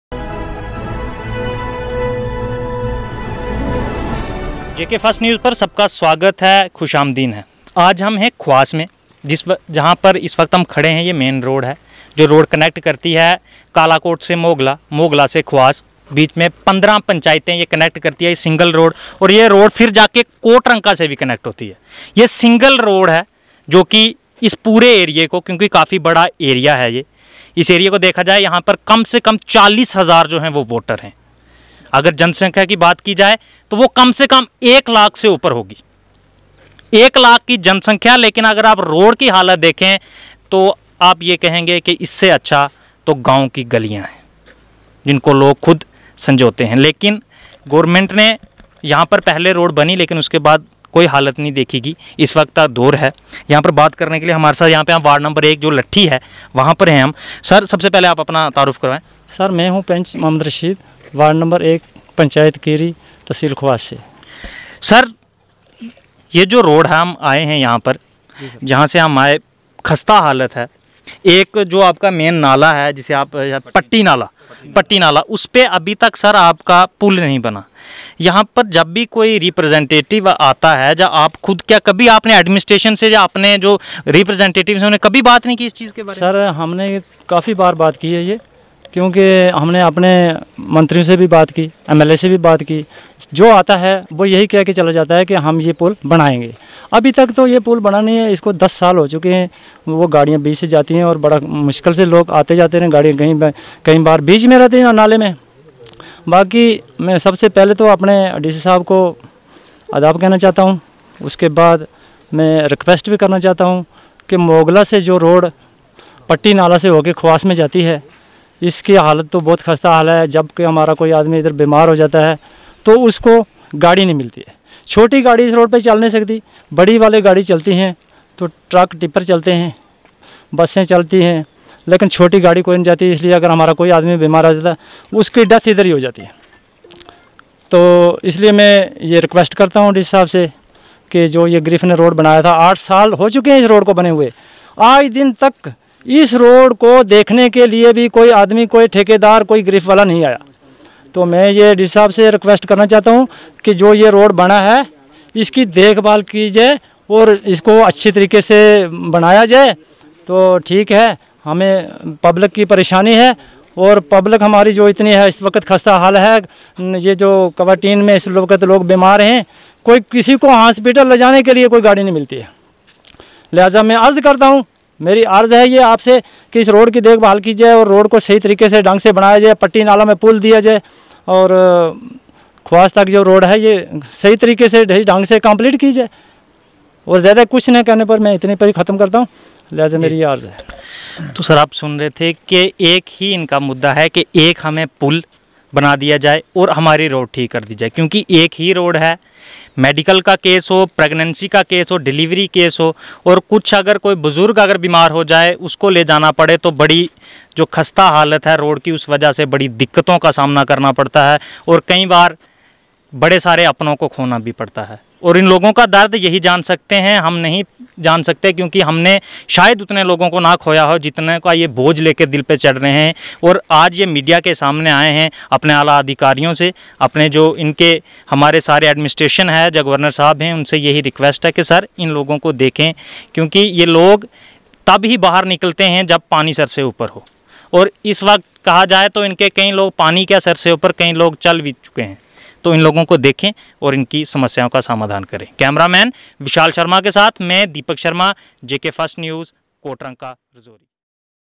JK First News,People's from khawas protest against various department in main market khawas rajour
People's from khawas protest against various department in main market khawas rajouri.
Repoter- Deepak Sharma
cameraman -Vishal Sharma